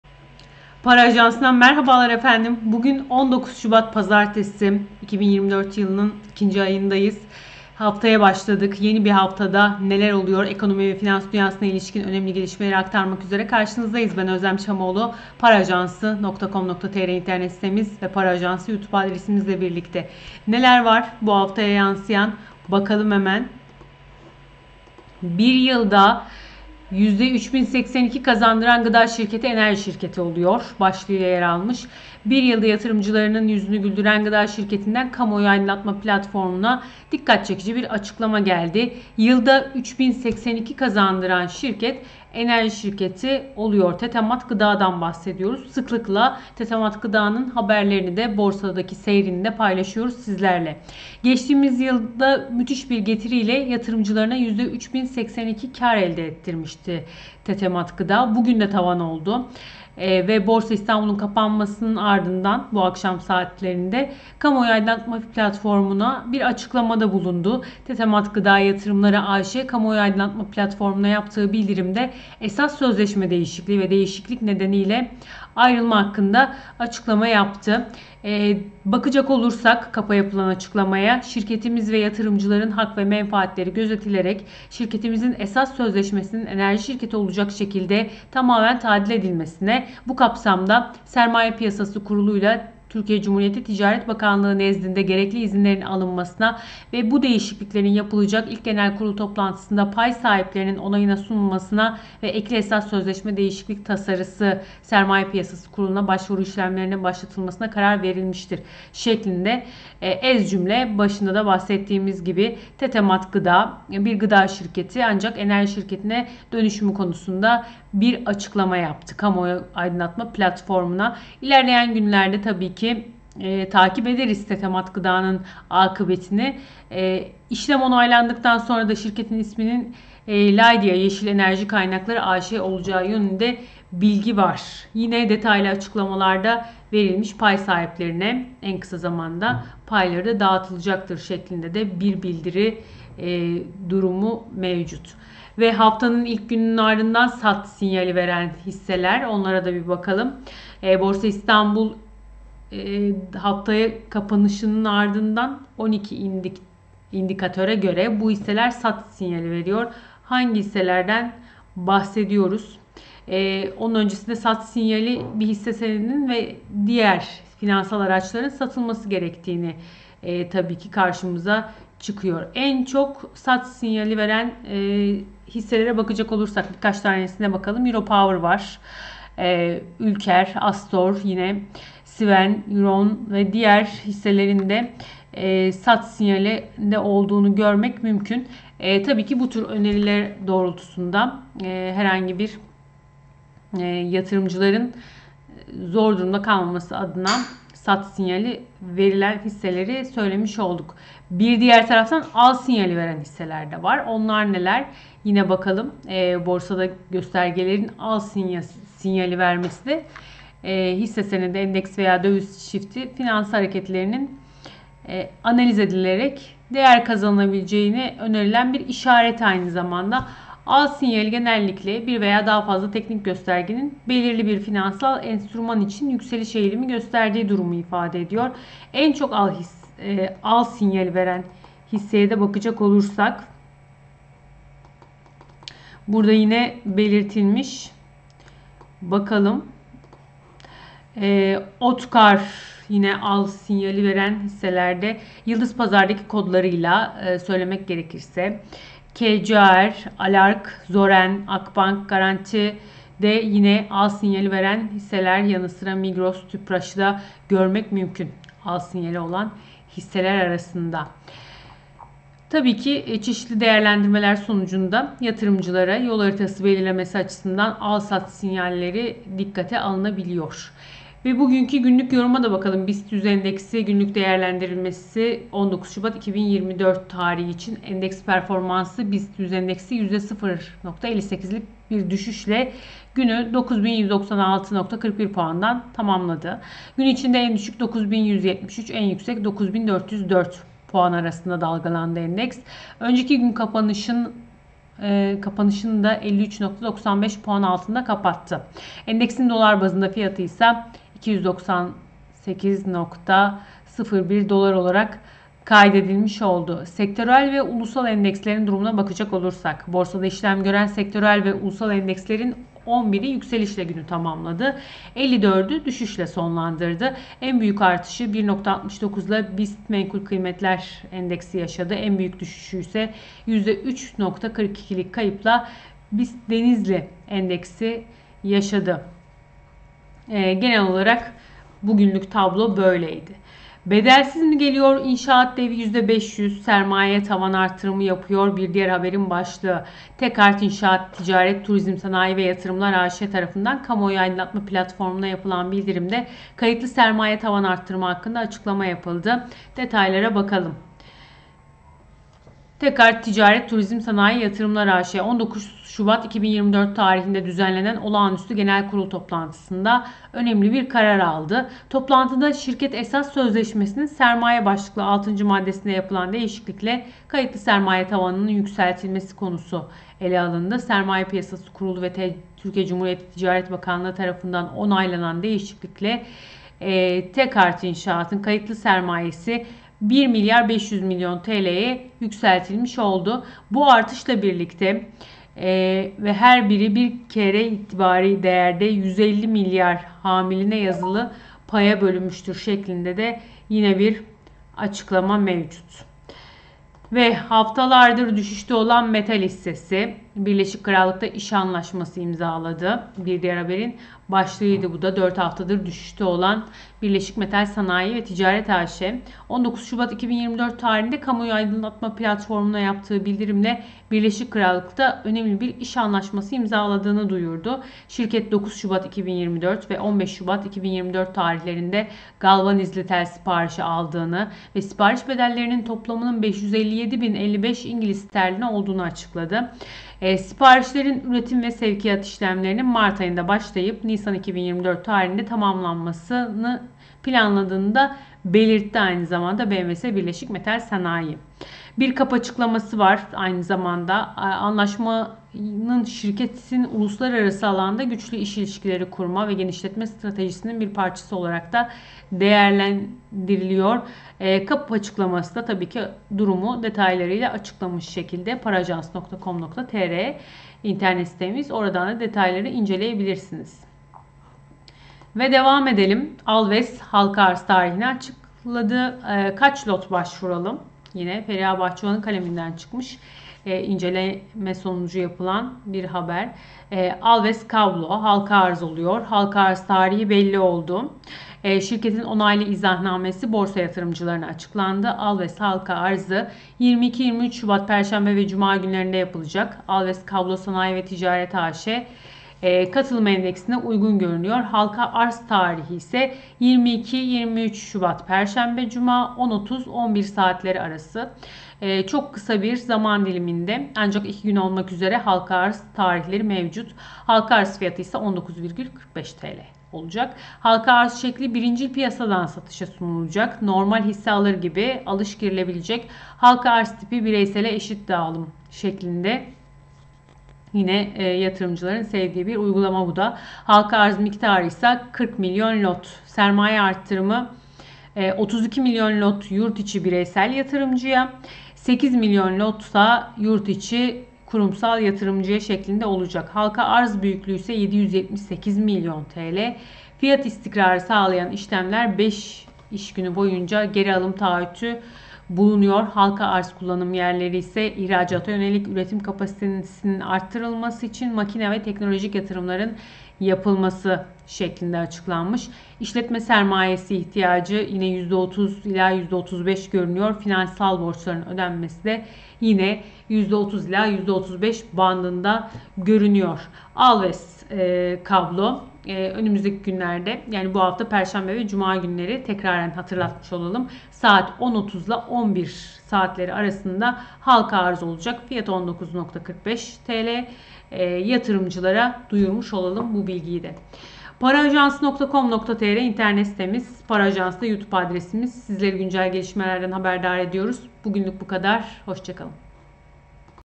19 ŞUBAT 2024 TARİHLİ EKONOMİ VE FİNANS DÜNYASINA İLİŞKİN GELİŞMELER
Bir Yılda %3082 Kazandıran Gıda Şirketi Enerji Şirketi Oluyor!
ALVES Halka Arz Tarihi Açıklandı!
Haftanın İlk Gününün Ardından "Al Sinyali" Veren Hisseler!